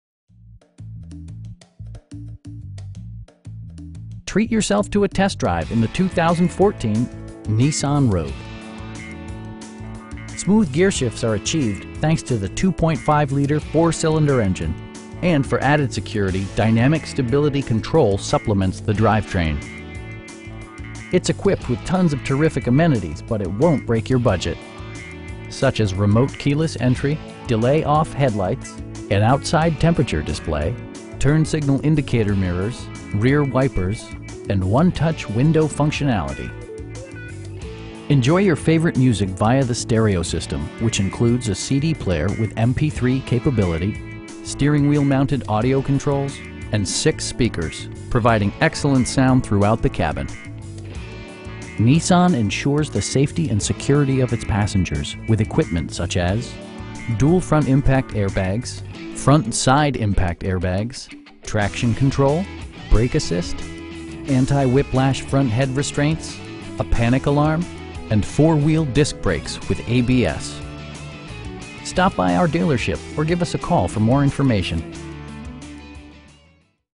2014 Nissan Rogue SV in Fayetteville, AR 72703
Superior Nissan
3372 North College Avenue

Treat yourself to a test drive in the 2014 Nissan Rogue. Smooth gearshifts are achieved thanks to the 2.5 liter 4 cylinder engine, and for added security, dynamic Stability Control supplements the drivetrain. It's equipped with tons of terrific amenities, but it won't break your budget. Such as remote keyless entry, delay-off headlights, an outside temperature display, front bucket seats, turn signal indicator mirrors, power windows, rear wipers, and 1-touch window functionality. Nissan ensures the safety and security of its passengers with equipment such as: dual front impact airbags, front SIDE impact airbags, traction control, brake assist, anti-whiplash front head restraints, a panic alarm, and 4 wheel disc brakes with ABS. We pride ourselves in the quality that we offer on all of our vehicles. Stop by our dealership or give us a call for more information.